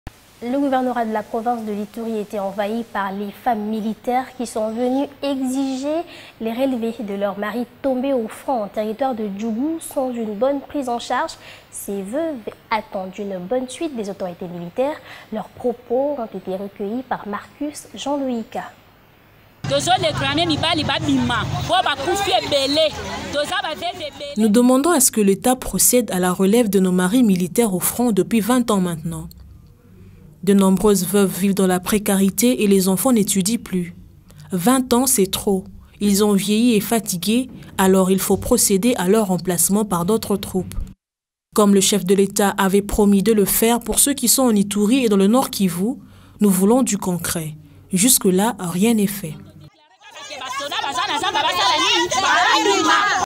Ituri: Les femmes de militaire réclament la relève de leurs maris au front
JT des provinces Perle Mbiya Lundi 03 Juin: Ituri, Les femmes de militaire réclament la relève de leurs maris au front